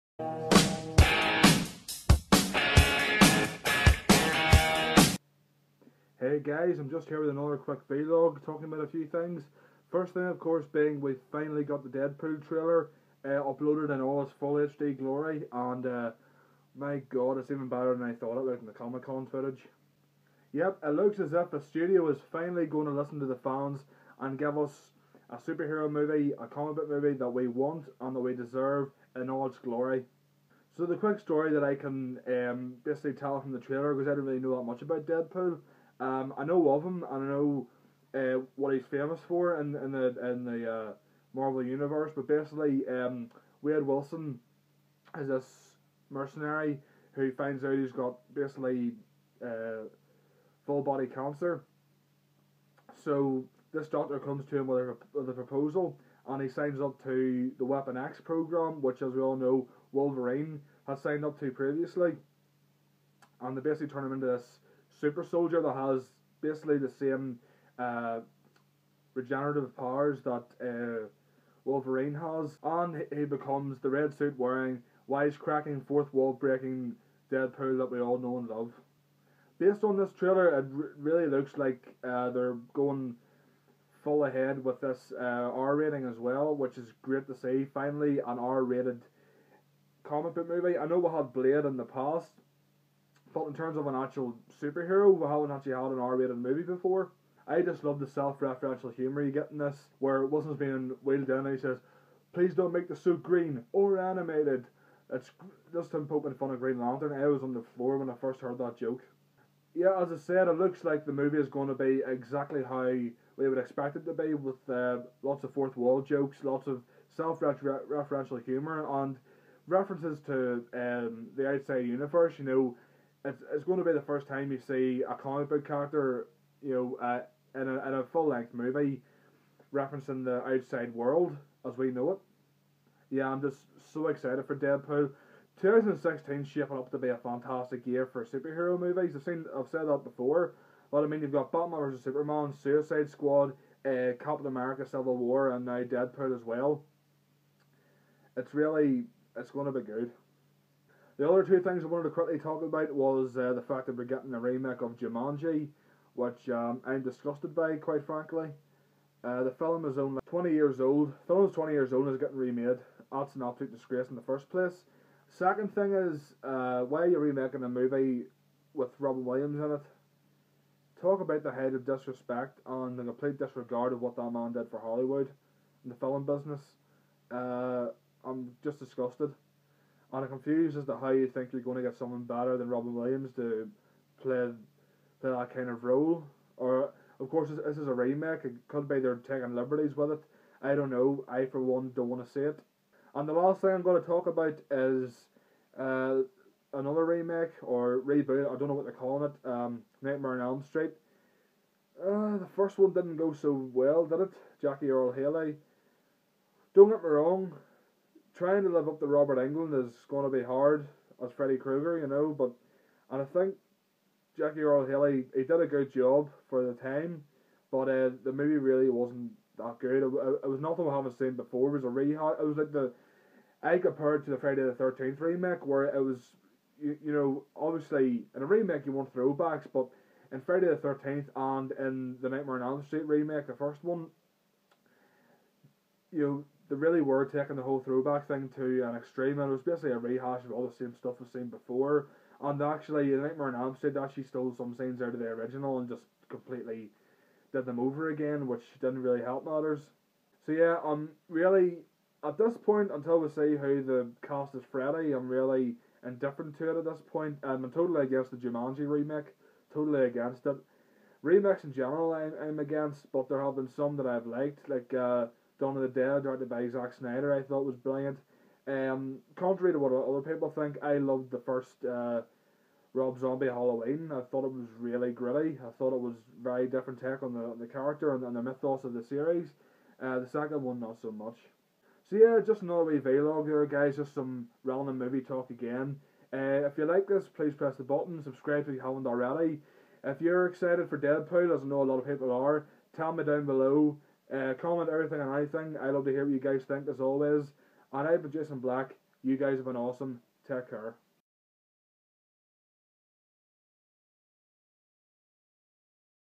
VLOG #4: Deadpool, Remakes and More!
In this VLOG, I give my thoughts on the full Deadpool trailer and the news of some remakes.